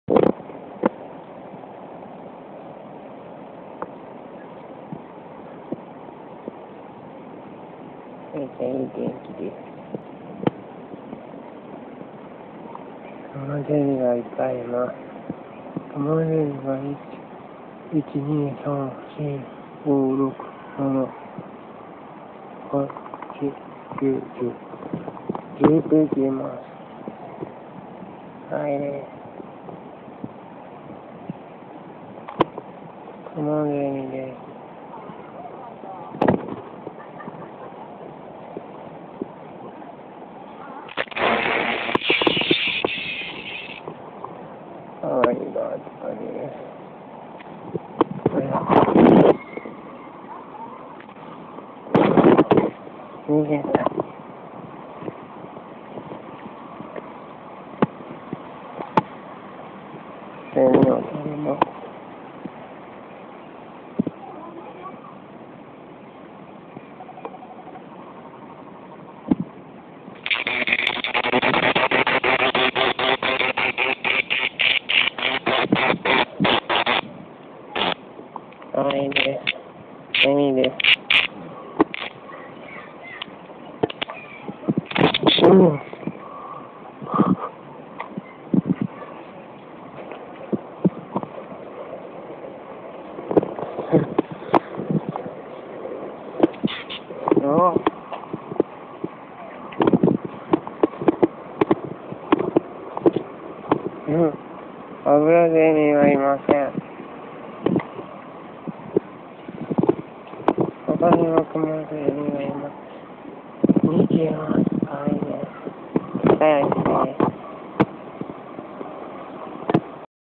神社にお参りに行ってセミと遊んだよ～＼(^o^)／♪ 2017.8/5 13:38:05
クマンゼミがぶりいっぱいおって、ぶり可愛いかった♡